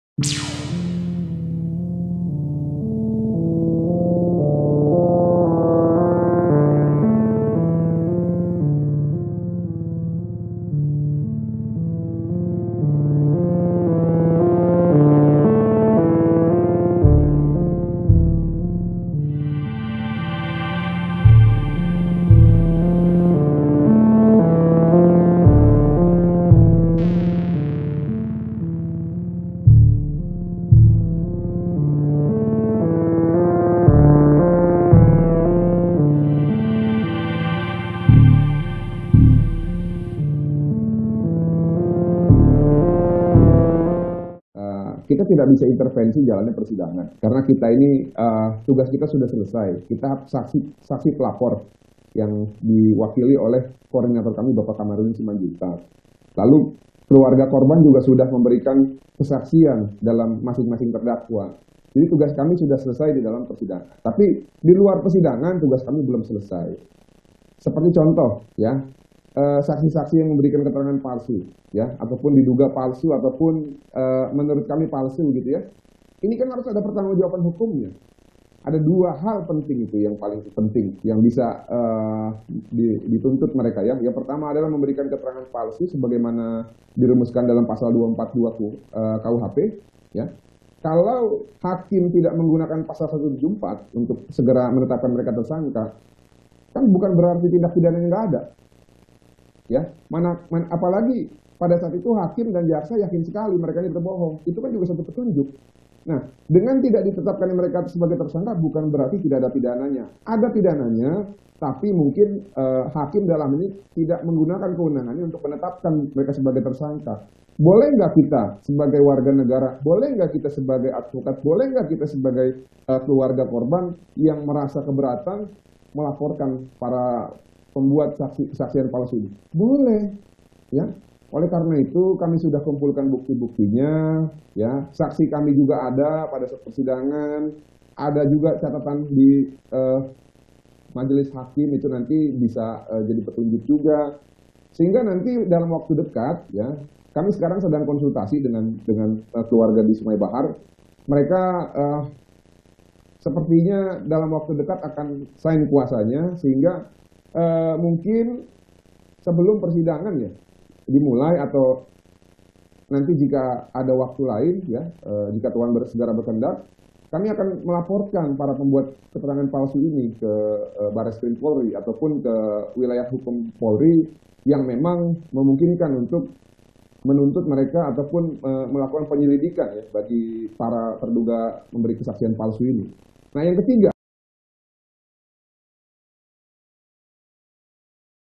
Kuasa Hukum Keluarga Yosua Ancam Pidanakan Saksi yang Beri Keterangan Palsu di Sidang
JAKARTA, KOMPAS.TV - Kuasa hukum keluarga Brigadir Yosua, Martin Lukas Simanjuntak berencana untuk melaporkan saks- saksi yang diduga memberikan keterangan palsu dalam persidangan kasus Ferdy Sambo dkk.

Martin optimis karena hakim pun sadar dengan kebohongan yang disampaikan saksi.

Meski begitu, Martin tidak menyebutkan nama saksi yang akan dilaporkan namun pihaknya tengah mempersiapkan laporan dan berkonsultasi dengan keluarga Yosua.

“Kami sudah kumpulkan bukti-buktinya, saksi kami juga ada saat persidangan ada juga catatan di majelis hakim, sehingga dalam waktu dekat, kami juga sekarang sedang konsultasi dengan keluarga di Sungai Bahar. Mereka dalam waktu dekat akan serahin kuasanya sehingga mungkin sebelum persidangan dimulai, jika ada waktu lain, jika Tuhan berkehendak. Kami akan melaporkan pembuatan keterangan palsu ini.” Kata Martin, pada Minggu (13/11/2022).

Menurut Martin, pihaknya sudah tidak bisa mengintervensi sidang, namun ia masih bisa menjawab argumentasi dan narasi yang dibentuk oleh kesaksian yang menyudutkan kliennya lewat media.